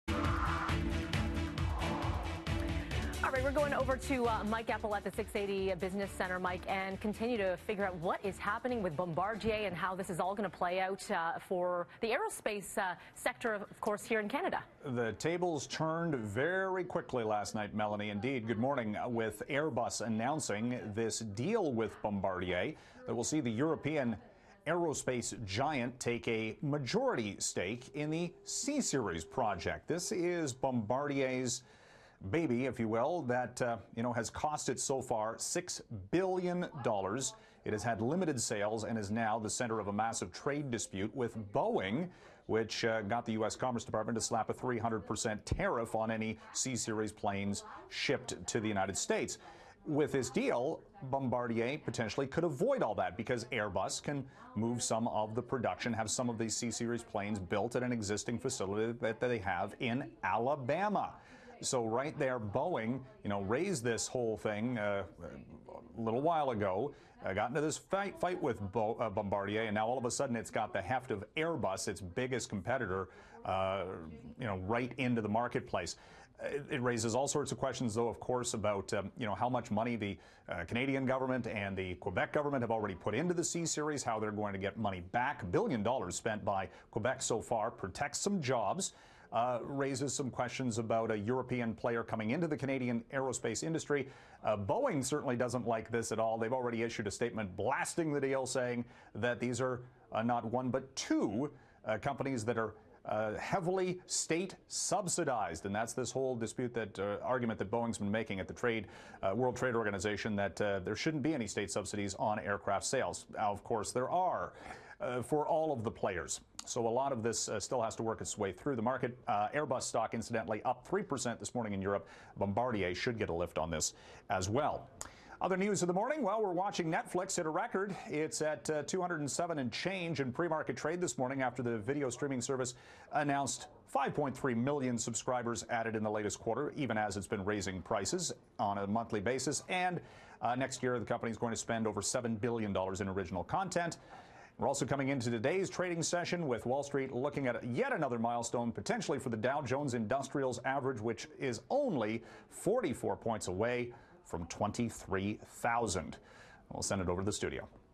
Airbus buys majority stake in Bombardier CSeries program
Bombardier sells a majority stake in the C-Series project and Netflix has hit a new record. 680 NEWS senior business editor Mike Eppel with the day's top business headlines.